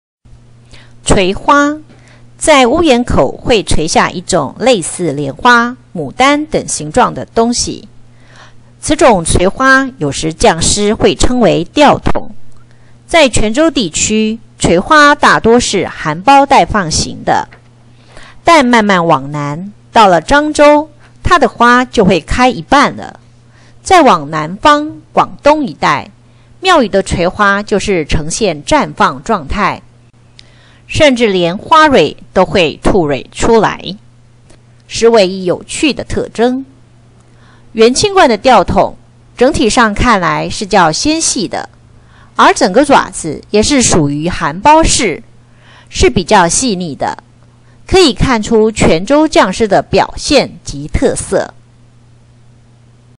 垂花
在屋簷口會垂下一種類似蓮花、牡丹等形狀的東西。此種垂花有時匠師會稱為「吊筒」，在泉州地區，垂花大多是「含苞待放型」的；但慢慢往南，到了漳州，它的花就會開一半了；再往南方、廣東一帶，廟宇的垂花就是呈現綻放狀態，甚至連花蕊都會吐蕊出來，實為一有趣的特徵。
元清觀的吊筒，整體上來看是較纖細的，而整個爪子也是屬於「含苞式」。是比較細膩的，可以看出泉州匠師的表現及特色。